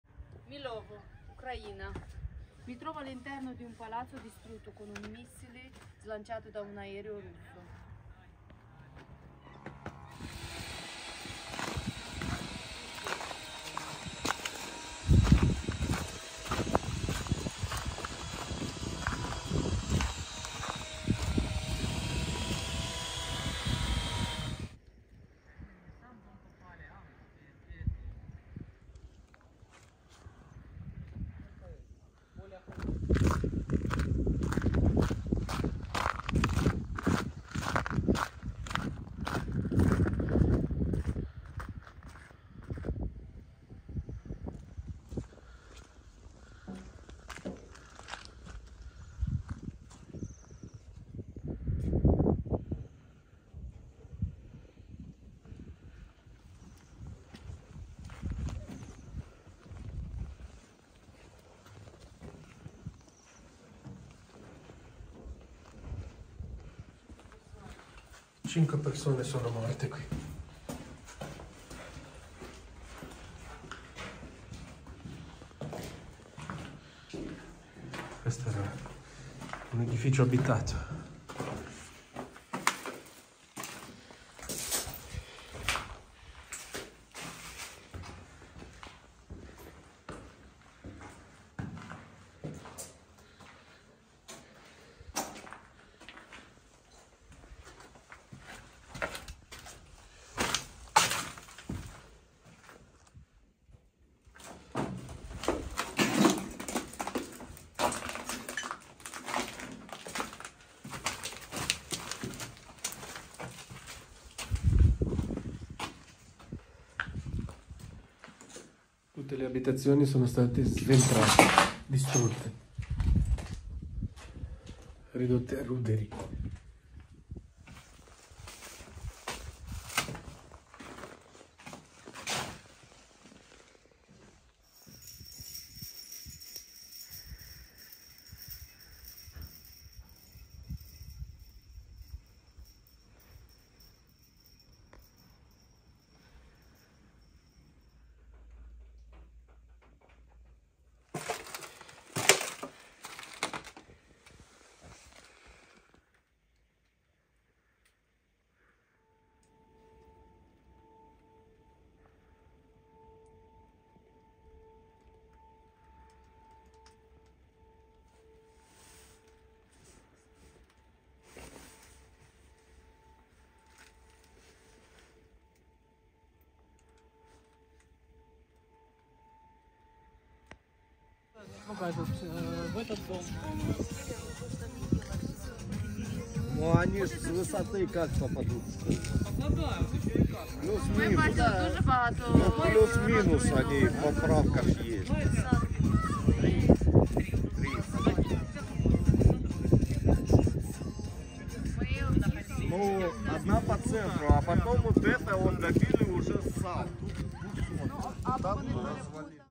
L'ultimo brutale attacco russo contro Milova
In questo video i nostri corrispondenti Alla Perdei e Giorgio Provinciali documentano gli effetti dell’ultimo brutale attacco aereo russo contro Milova, piccolo villaggio dell’oblast’ di Kharkiv già devastato dai colpi d’artiglieria durante la battaglia per liberarlo dall’occupazione rascista.